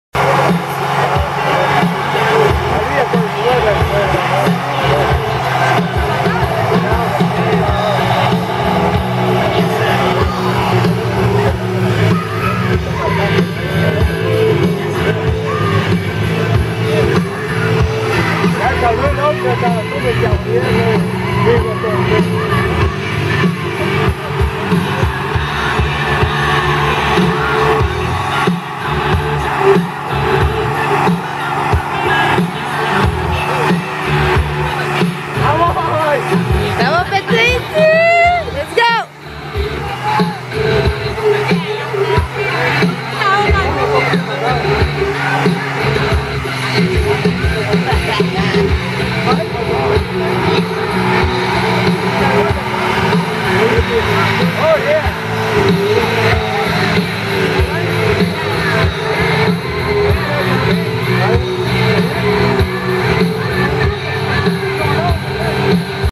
SHOW DE BOUCANE DU BLOU DRAG
Show de boucane du Blou Drag du 29 juin 2012 les autres dates des shows de boucane sont le 27 juillet, 3 août, 10 août et le 24 août a 19h. Pour tout les détails